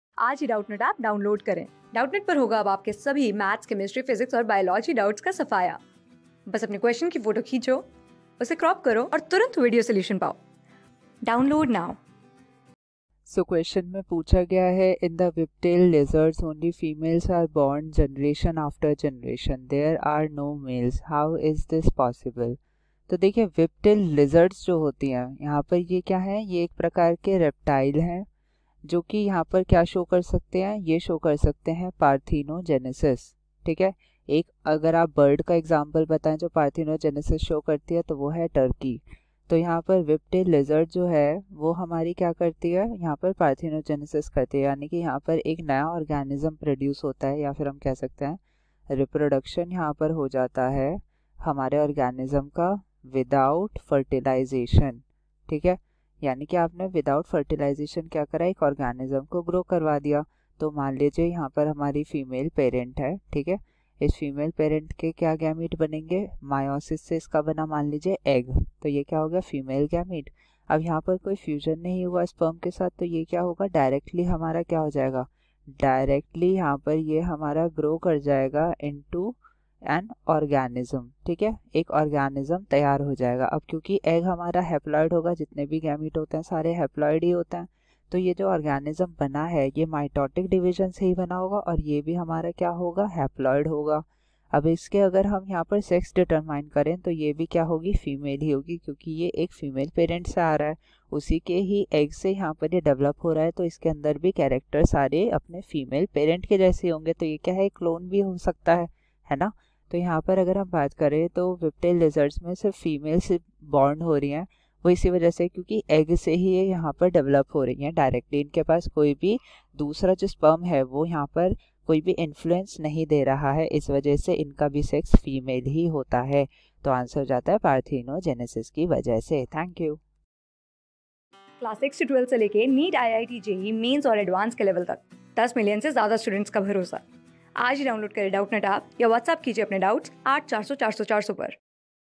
In the whiptail lizards only females are born generation after generation. There are no males.
In the whiptail lizards only females are born generation after generation. There are no males. How is this possible ?